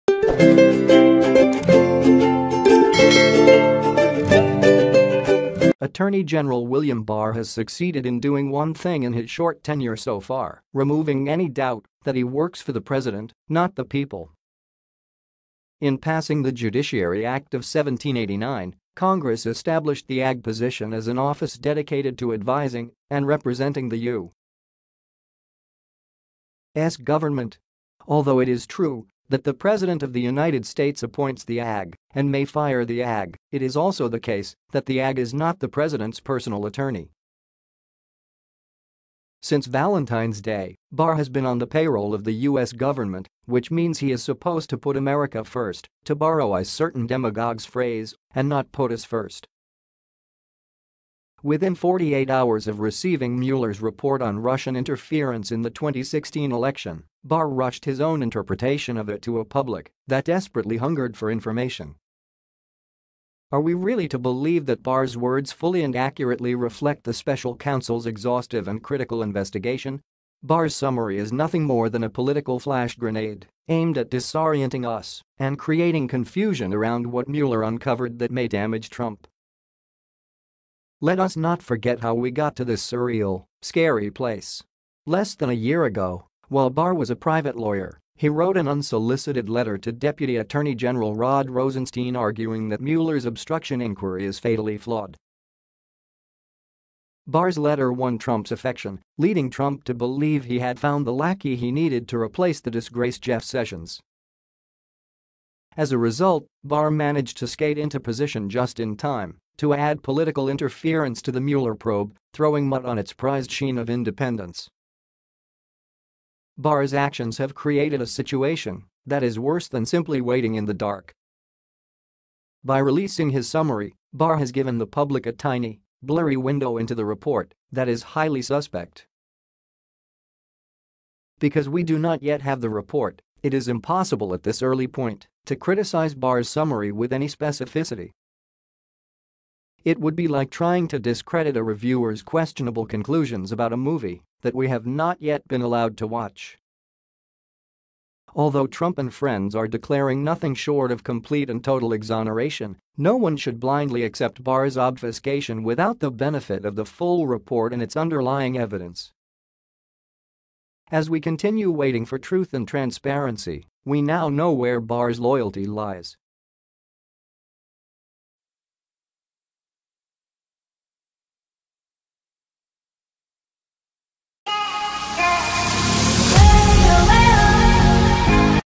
Trump and his team is now sitting on a ticking timebomb
Attorney General William Barr has succeeded in doing one thing in his short tenure so far: removing any doubt that he works for the president, not the people.  In passing the Judiciary Act of 1789, Co